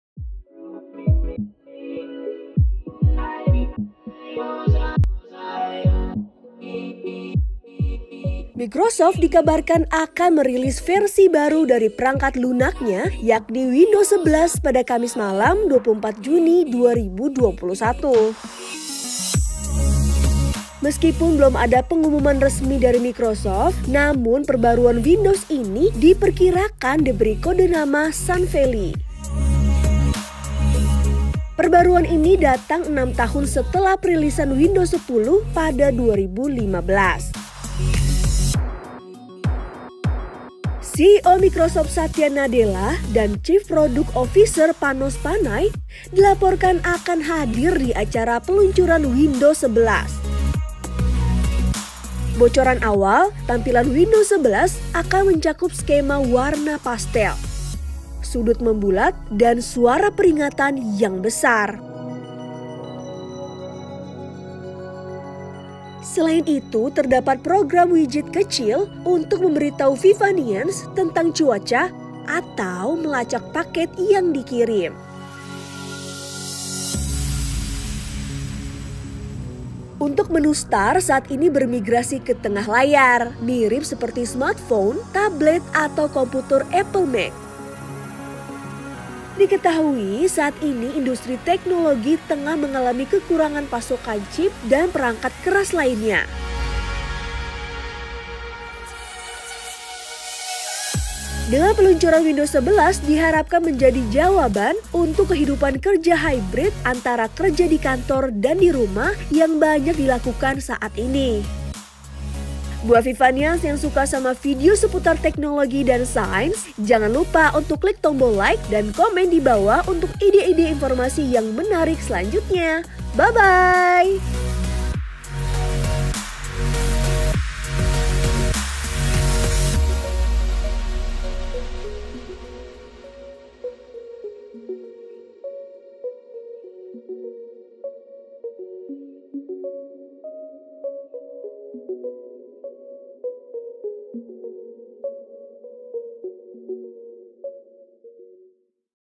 Microsoft Siap Rilis Windows 11!
VIVA - Microsoft dikabarkan akan merilis versi baru dari perangkat lunaknya,  yakni windows 11 pada Kamis malam 24 juni 2021. Perbaruan ini datang enam tahun setelah perilisan Windows 10 pada 2015.